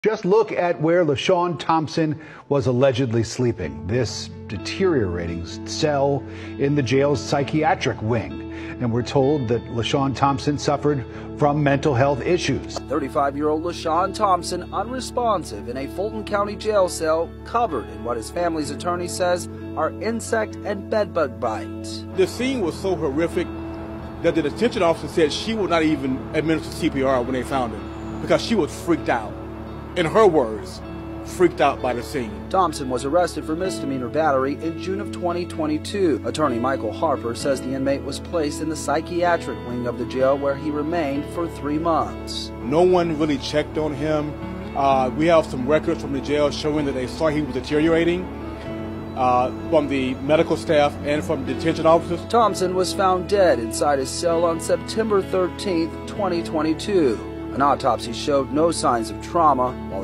Man dies in Fulton County jail from bed bug infestation.
Man dies in Fulton county jail in Atlanta, Georgia from bedbug infestation.
#￼atlanta